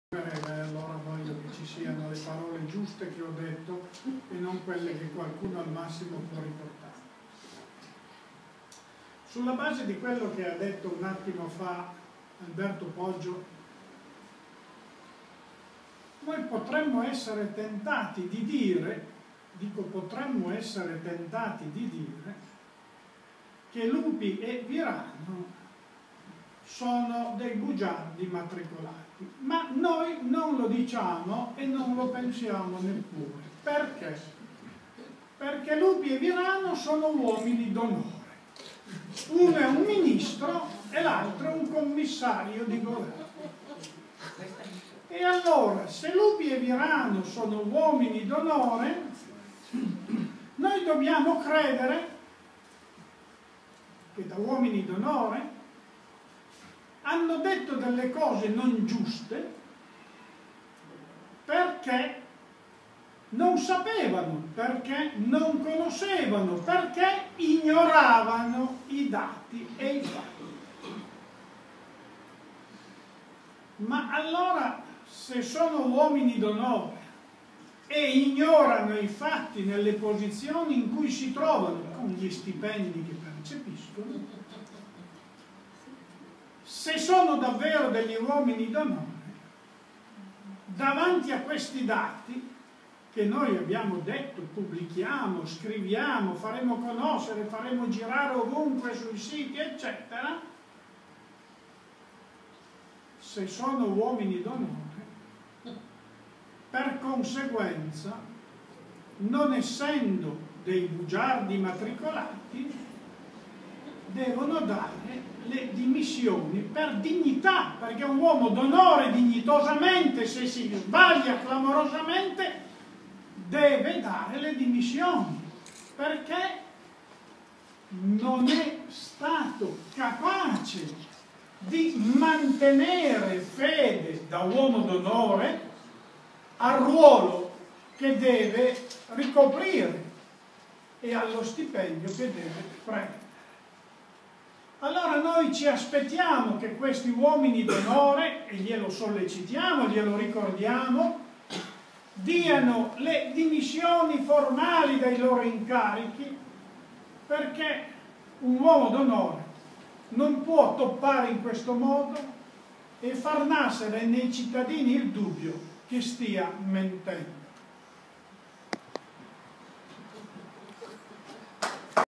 Alberto Perino sulle dichiarazioni del Min. Lupi e il comm. Virano
Alberto Perino alla conferenza stampa notav ormaietardi sulle dichiarazioni del Min. Lupi e il comm. Virano . Tutti i materiali qui